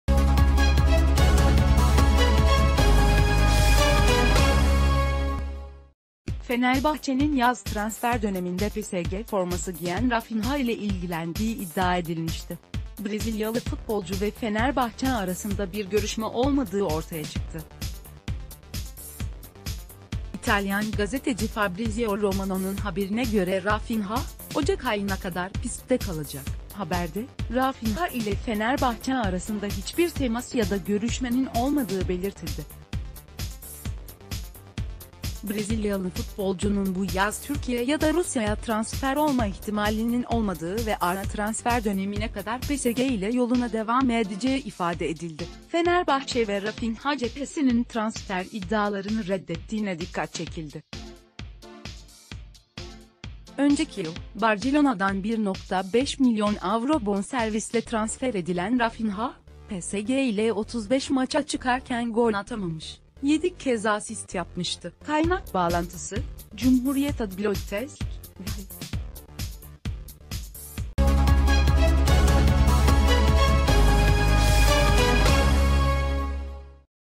Fenerbahçe'nin Rafinha ile ilgilendiği iddia edildi
Fenerbahçe'nin yaz transfer dö...